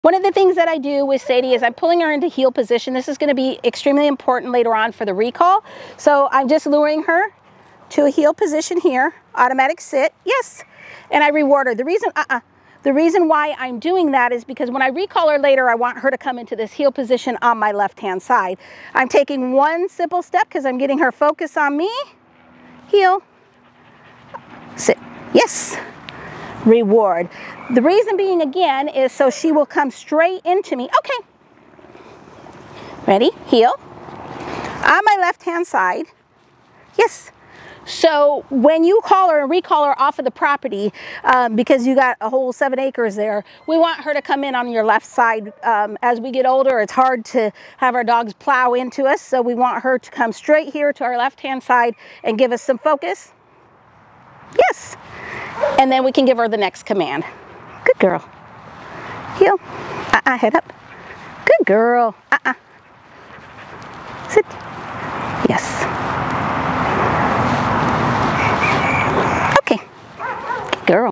Working with Sadie on mastering the heel position
Working with Sadie on mastering the heel position is key to her recall training. By luring her into position and rewarding her focus, we're teaching her to come straight to my left side on command. This will ensure she responds consistently, no matter where she is, and avoids plowing into us. Heel, sit, and focus—Sadie’s getting it down like a pro! 🐾💪